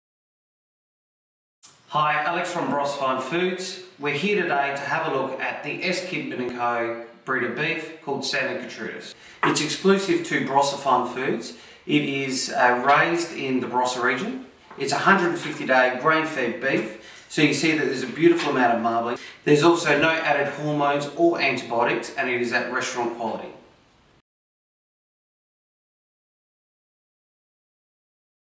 Kidman Santa Gertrudis Beef
We're excited to introduce Kidman Santa Gertrudis Beef, now available in our stores!
This premium SA raised beef is free of added hormones and antibiotics and the cattle are grain fed and graded to ensure premium eating quality.
With high standards of animal welfare, we’re proud to be the exclusive stockists of this iconic South Australian brand!